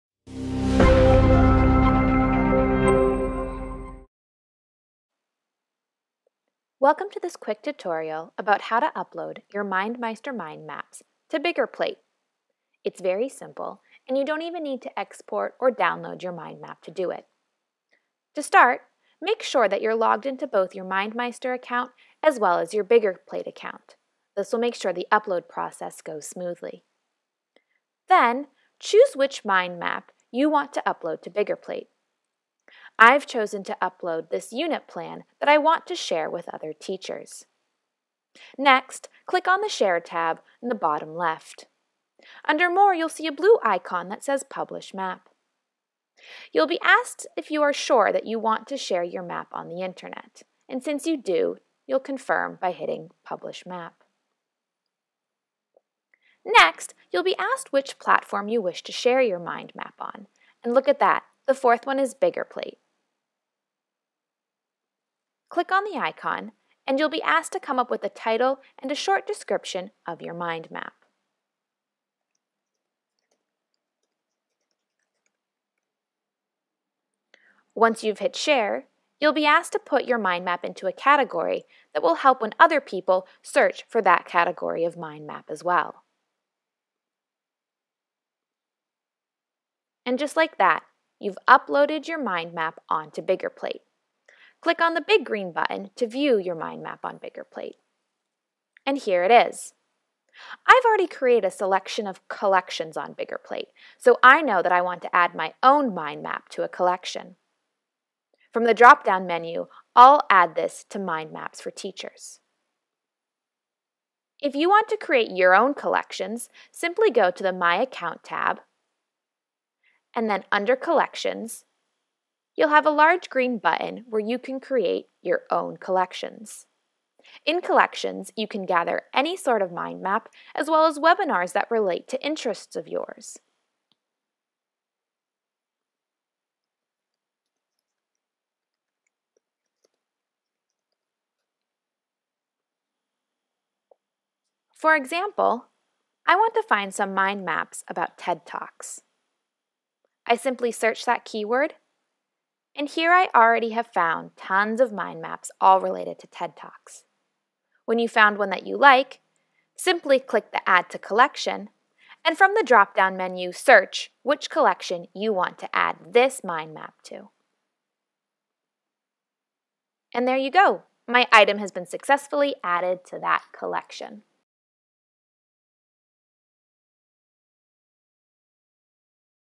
MindMeister Tutorial: Sharing your maps on Biggerplate.com
Here is a quick run-through of how to upload your MindMeister mind maps directly to Biggerplate. With the built-in Biggerplate integration feature, you don’t need to download or export your mind maps first. With a few quick clicks, you can easily share your mind map creations with a bigger audience. Easily add your mind maps to collections to share with other creators.